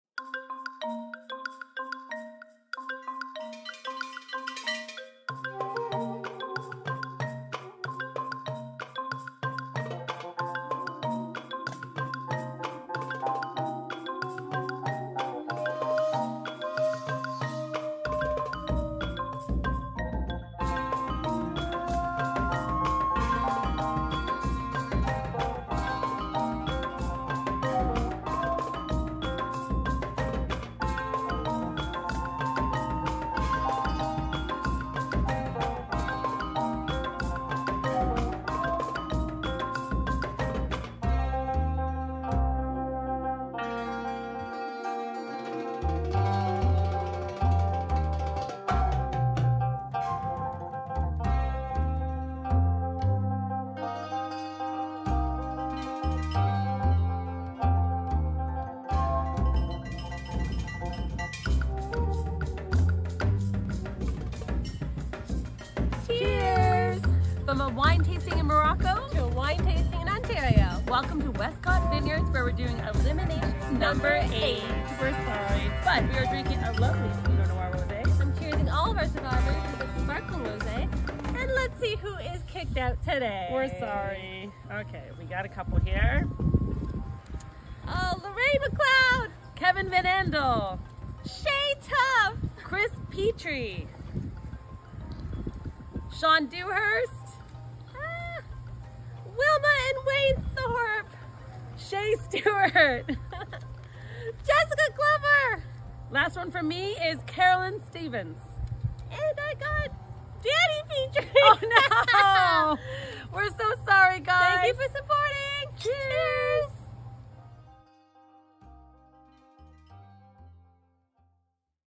H4H Elimination #8 - Liquor Cabinet Edition - Wine Tasting
We go from wine tasting in Morocco to wine tasting back in Ontario and both wines were delicious! Although we didn't have a token camel in our backyard to help pick the names this time, we were still able to eliminate a few. Was it you? Watch and find out! A&K